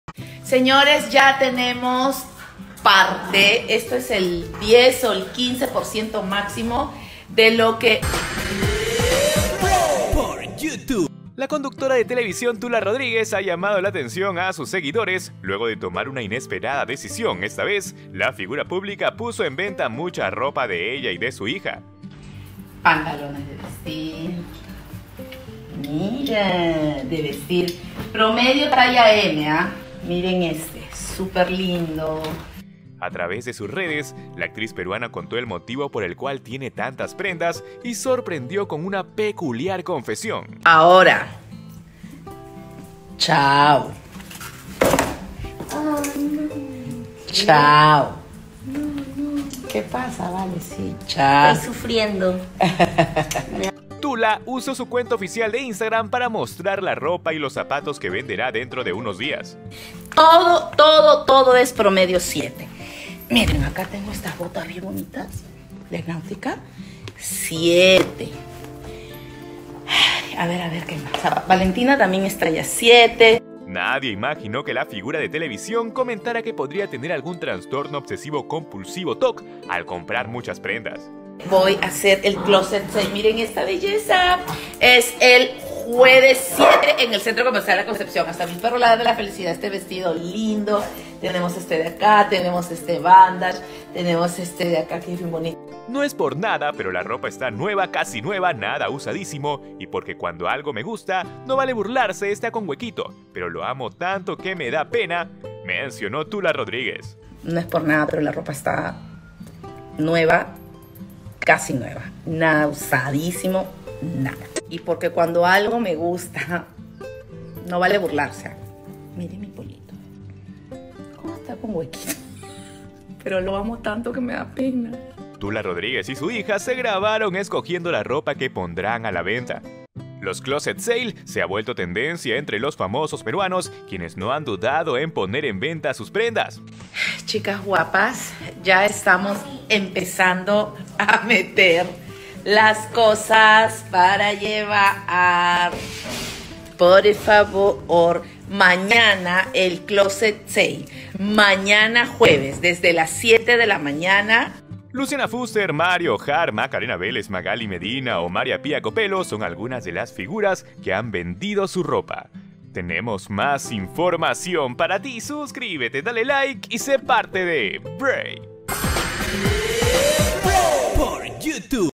TULA RODRIGUEZ VENDE SU ROPA Y LA DE SU HIJA PARA JUNTAR DINERO ¿QUÉ PASÓ?
Tula Rodriguez compartió con sus seguidores las prendas que colocará en venta.